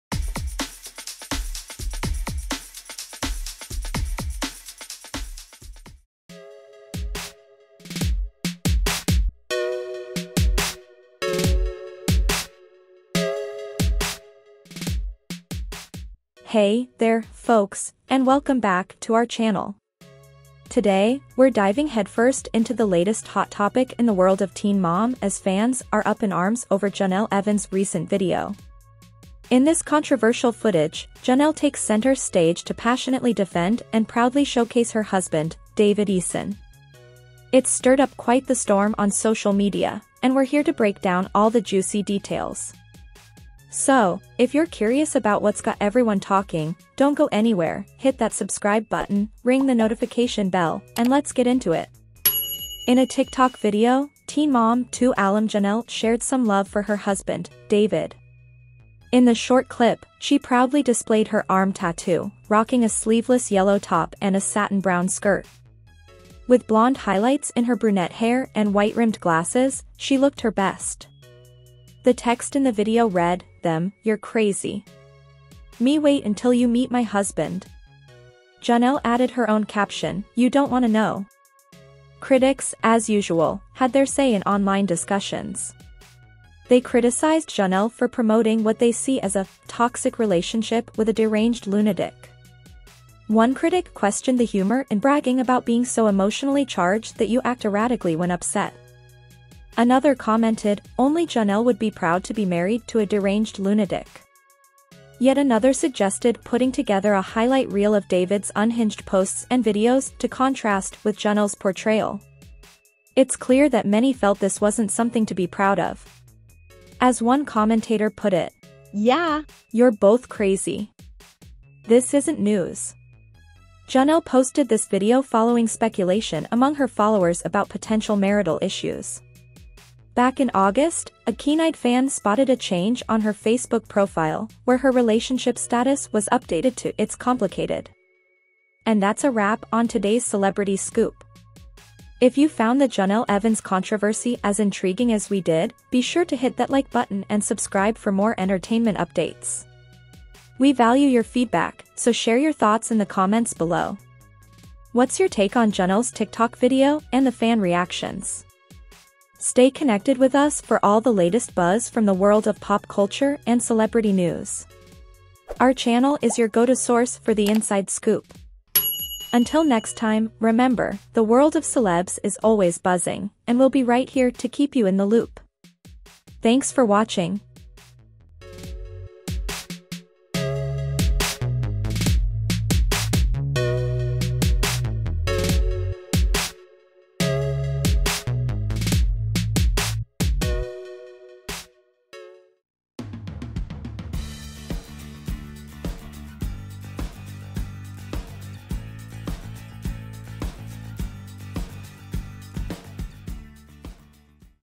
'Teen Mom' Jenelle Evans Drops Bombshell About Her 'Crazy' Marriage!
Today, we're diving headfirst into the latest hot topic in the world of Teen Mom as fans are up in arms over Jenelle Evans' recent video. In this controversial footage, Jenelle takes center stage to passionately defend and proudly showcase her husband, David Eason. It's stirred up quite the storm on social media, and we're here to break down all the juicy details.